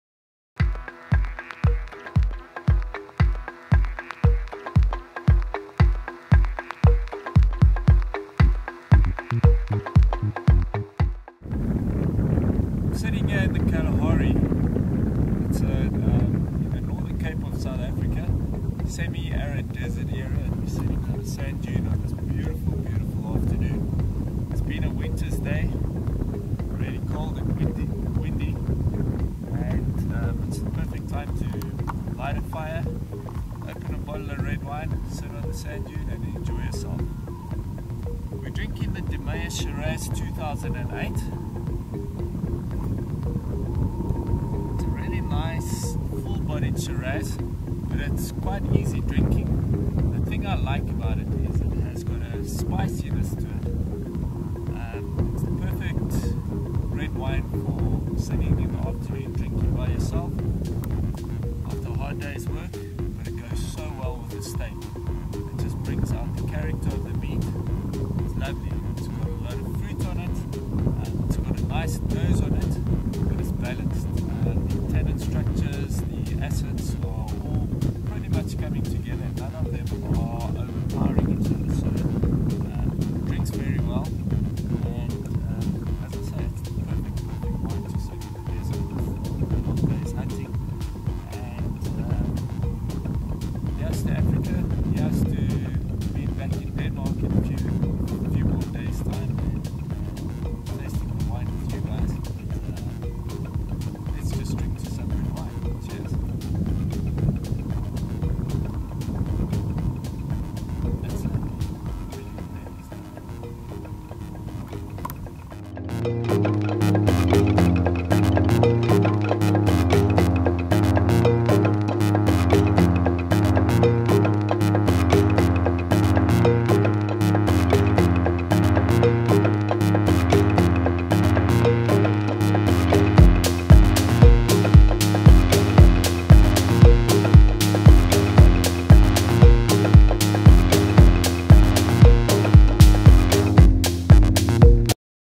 Hunting in the Kalahari & Tasting De Meye Shiraz
Mathew Castle tasting De Meye Shriaz in the Kalahari Desert